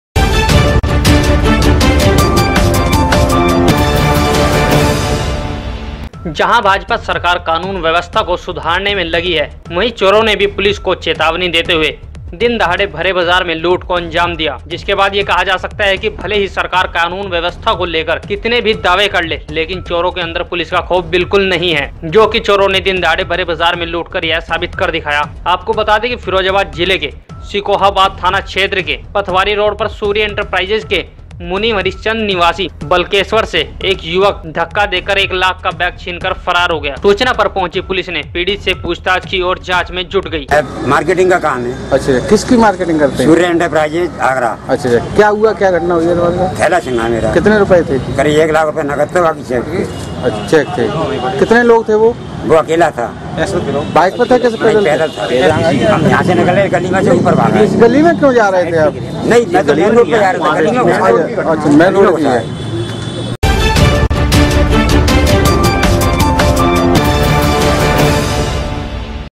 दिन दहाड़े एक लाख की लूट
( क्राइम स्टोरी के लिए उत्तर प्रदेश के फिरोजाबाद से जाहिद हुसैन की रिपोर्ट I )